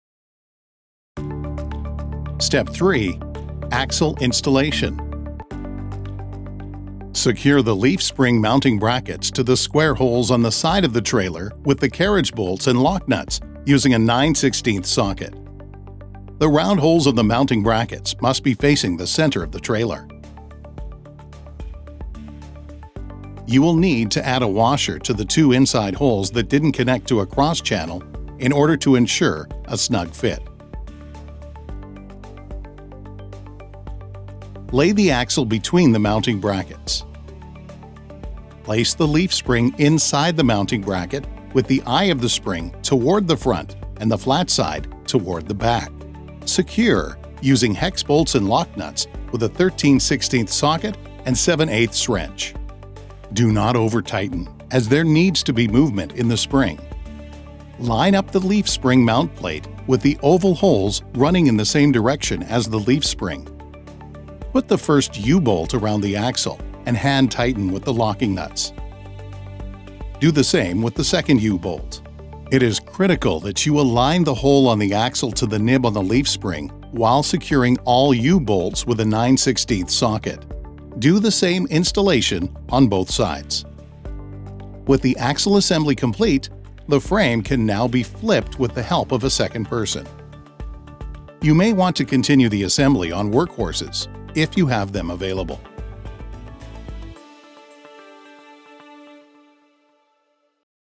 Step 3 - Axle Installation
Secure the leaf-spring mounting-brackets to the square holes on the trailer's side with the carriage-bolts and lock-nuts using a nine/sixteenth socket.

The round holes of the mounting-brackets must be facing the center of the trailer.

You will need to add a washer to the two inside holes that didn’t connect to a cross-channel to ensure a snug fit.

Lay the axle between the mounting-brackets.

Place the leaf-spring inside the mounting-bracket with the spring's eye toward the front and the flat side toward the back.

Secure using hex-bolts and lock-nuts with a thirteen/sixteenth socket and seven/eighths wrench.

Do not over-tighten as there needs to be movement in the spring.

Line up the leaf-spring mount plate with the oval holes running in the same direction as the leaf-spring.

Put the first u-bolt around the axle and hand-tighten with the locking nuts. Do the same with the second u-bolt.

You must align the hole on the axle to the nib on the leaf-spring while securing all u-bolts with a nine/sixteenth socket.

Do the same installation on both sides.

With the axle assembly complete, the frame can now be flipped with a second person's help.

You may want to continue the assembly on workhorses if you have them available.